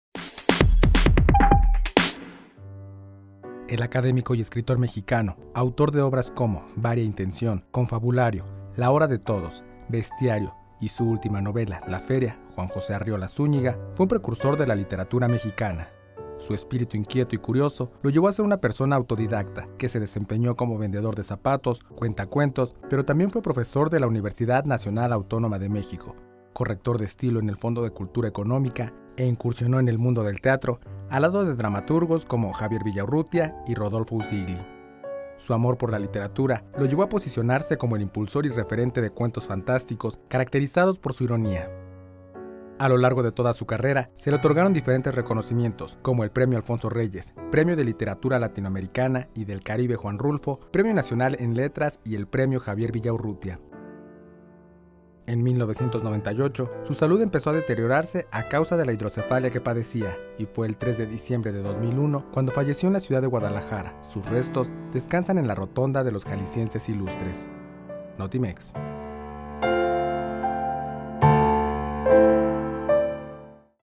Juan José Arreola, precursor de la literatura mexicana
Su amor por la literatura lo llevó a posicionarse como impulsor y referente de cuentos fantásticos, caracterizados por su ironía